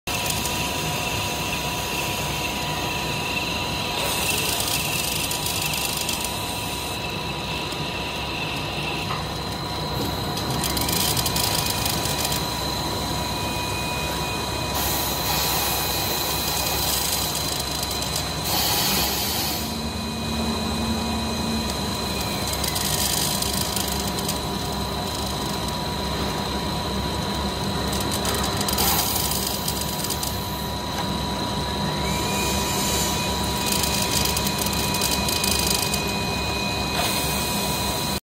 China Malex automatic cutting winder for bubble film machine
Automatic cutting winder with perforator for bubble film machine